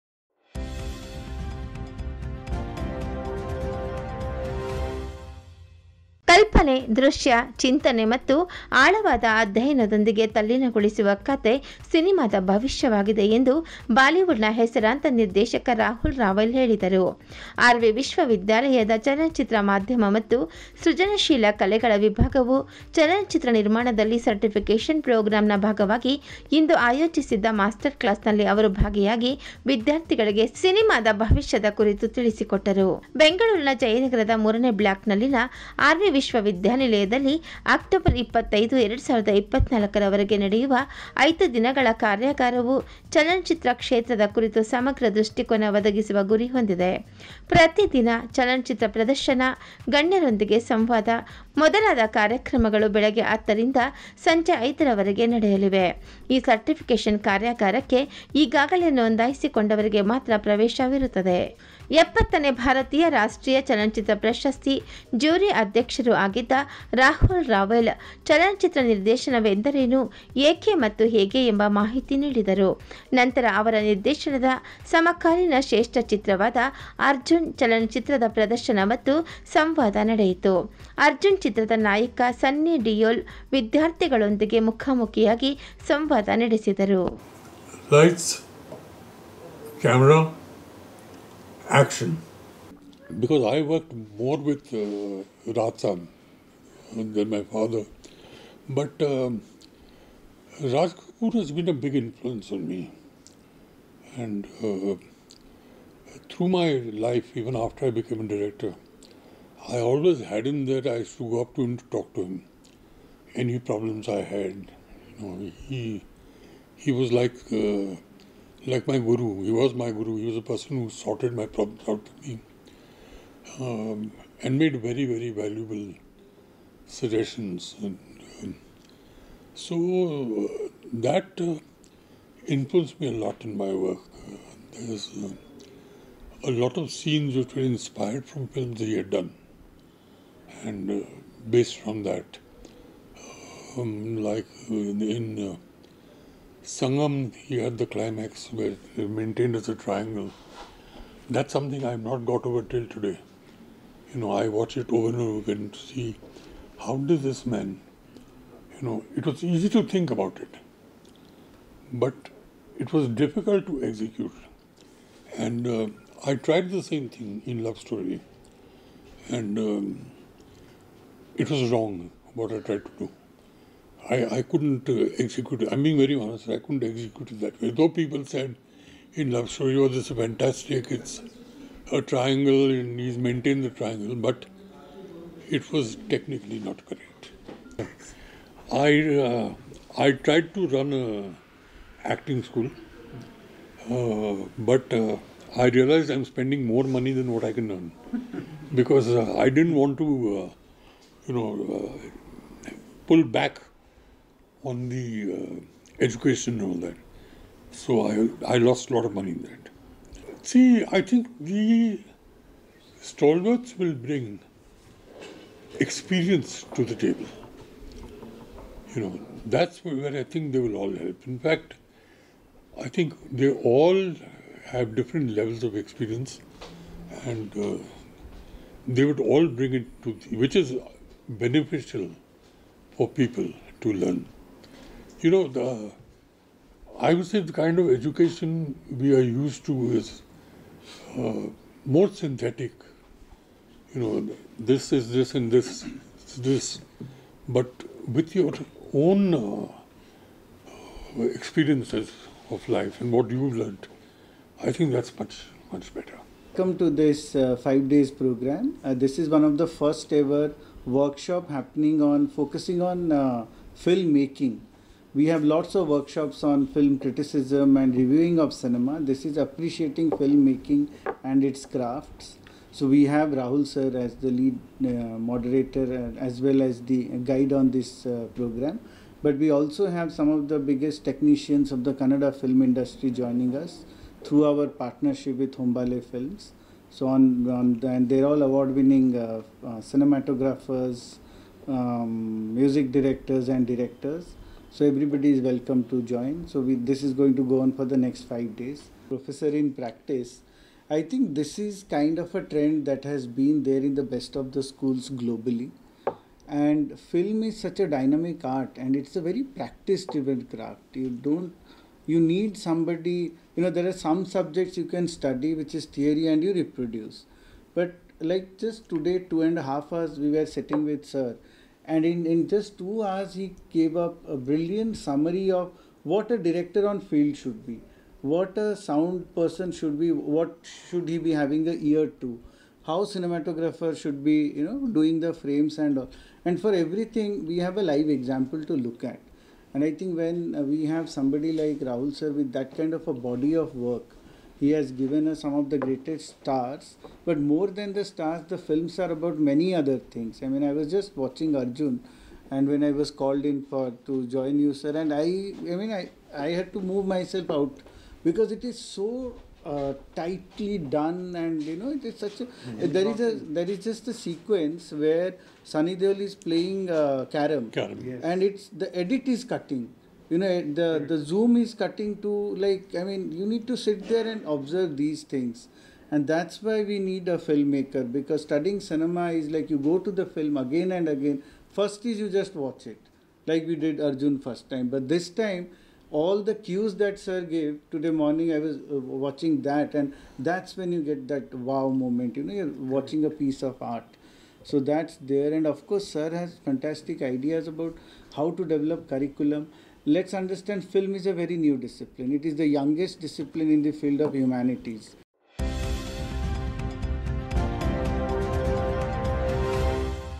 CERTIFICATE PROGRAM IN APPRECIATING FILM MAKING 21 25 OCTOBER 2024
ಐದು ದಿನಗಳ ಕಾರ್ಯಾಗಾರವು ಚಲನಚಿತ್ರ ಕ್ಷೇತ್ರದ ಕುರಿತು ಸಮಗ್ರ ದೃಷ್ಟಿಕೋನ ಒದಗಿಸುವ ಗುರಿ ಹೊಂದಿದೆ. ಪ್ರತಿ ದಿನ ಚಲನಚಿತ್ರ ಪ್ರದರ್ಶನ, ಗಣ್ಯರೊಂದಿಗೆ ಸಂವಾದ ಮೊದಲಾದ ಕಾರ್ಯಕ್ರಮಗಳು ಬೆಳಿಗ್ಗೆ 10ರಿಂದ ಸಂಜೆ 5ರವರೆಗೆ ನಡೆಯಲಿವೆ